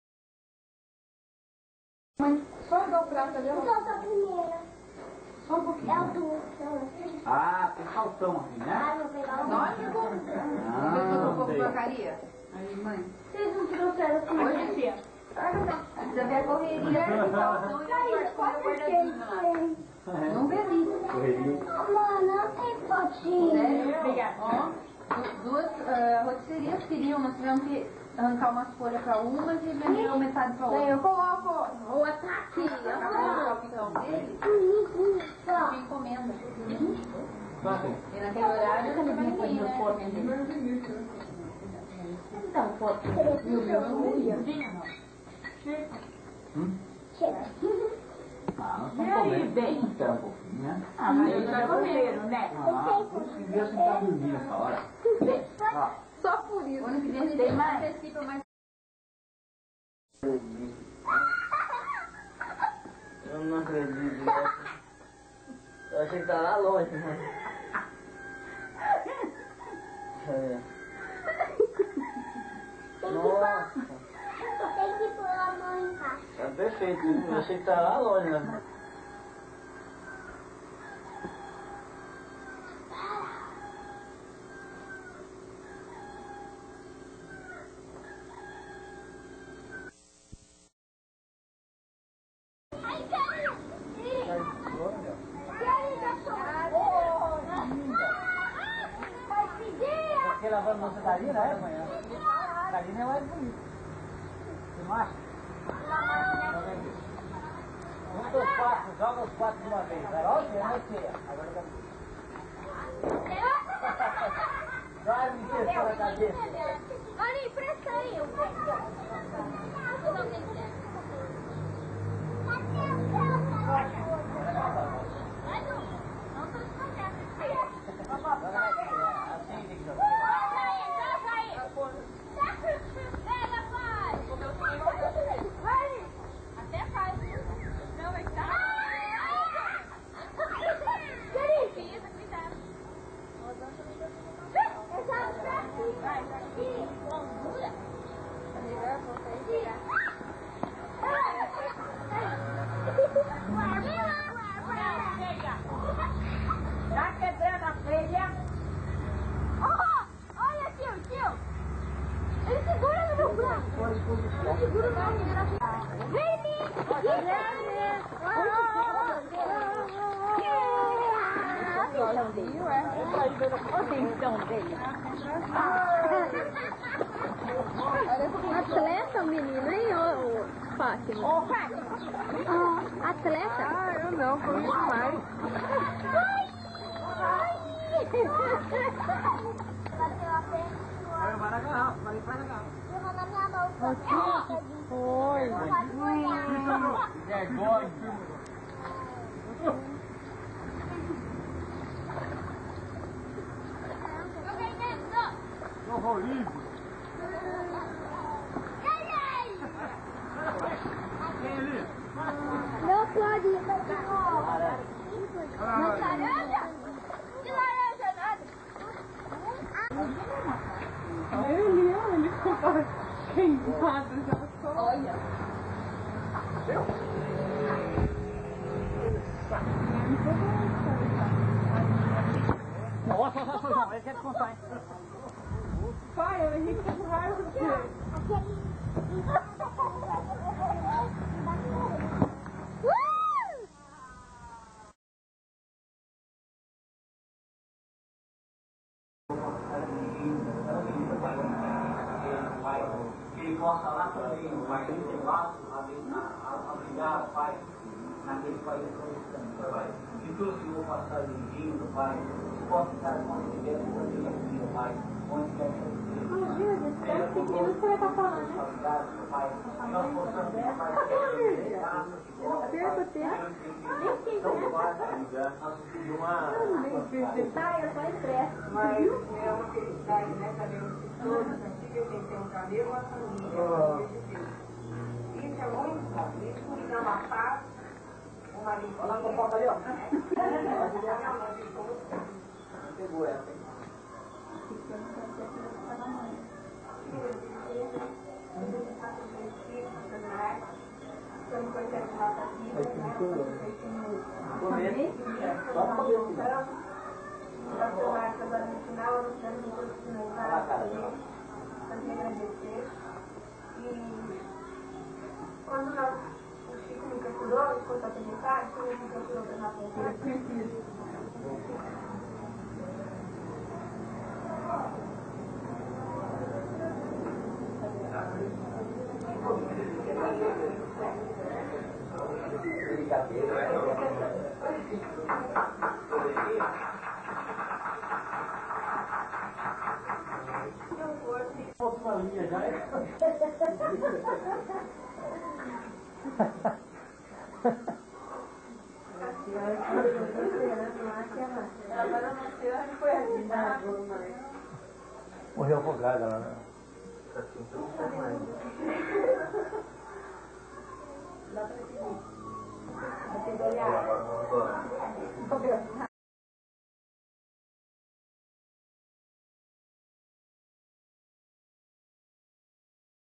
Video familiar - janeiro de 1992
ceia de final de ano, primeiro dia do ano, brincando com um dos presentes, assistindo o video do noivado do C&C (set.1991)